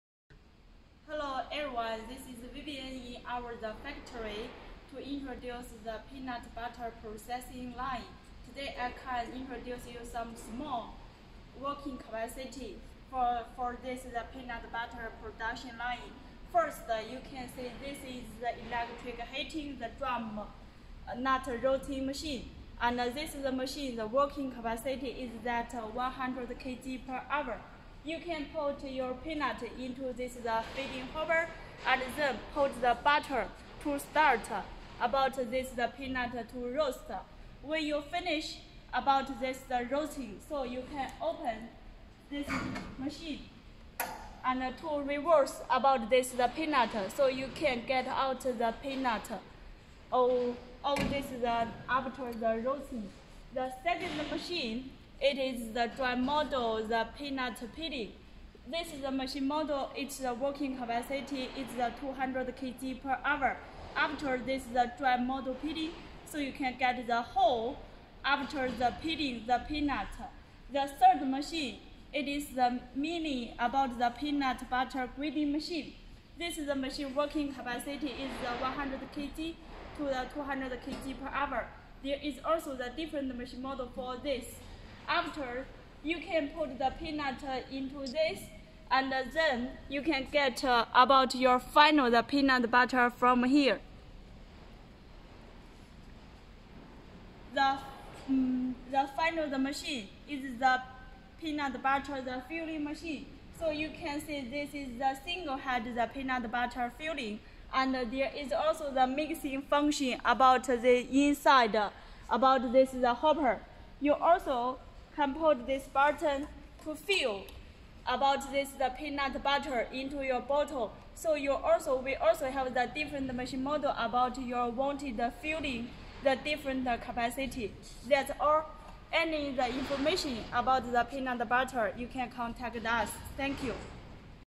The Introduce of Simple Peanut Butter Processing Line from Clover--008615516969050
The Processing Flow of Peanut Butter Processing:
Roasting---Peeling Grinding Filling.